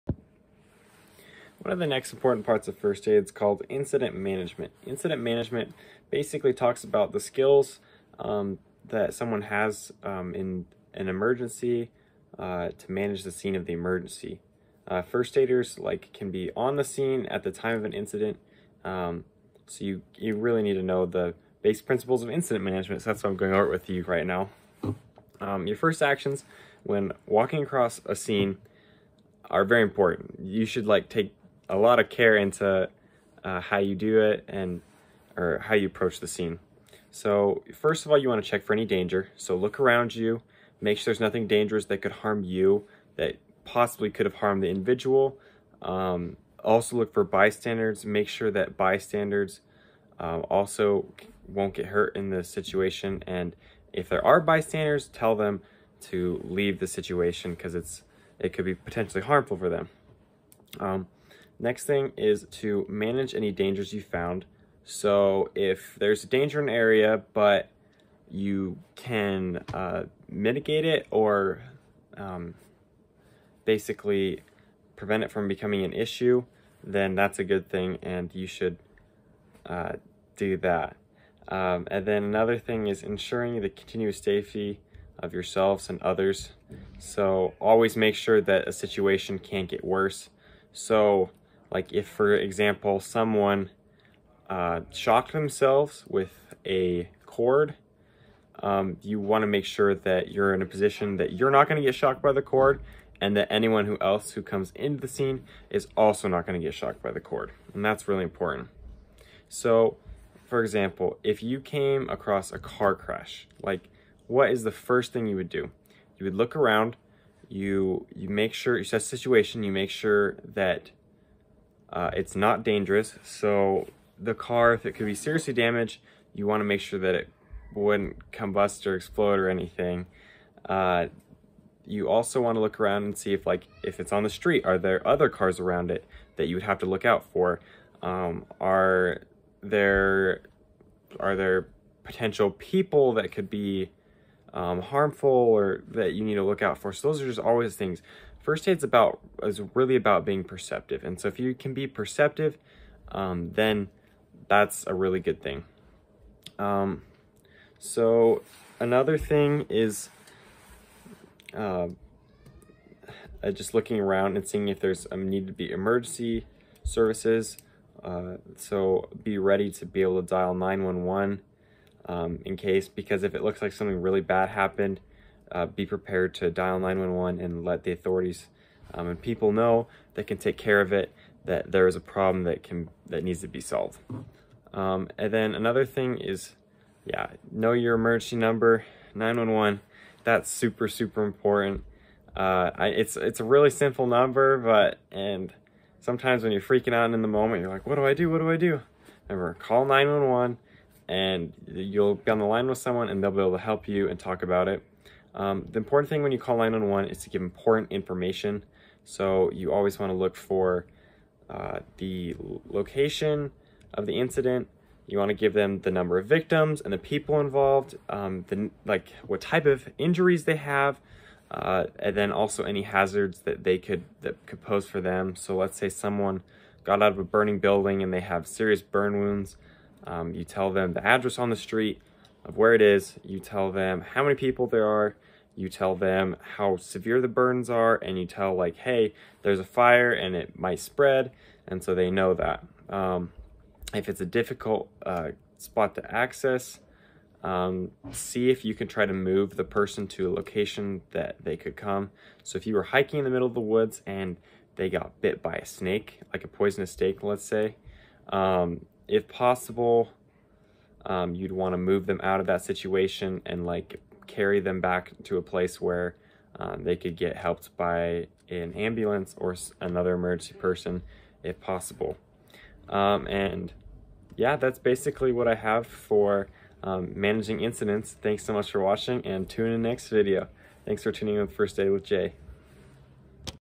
Managing the Scene (How to approach first Aid Scenarios)
Hello today I will be going over how to manage first aid situation and how you should always approach the scene.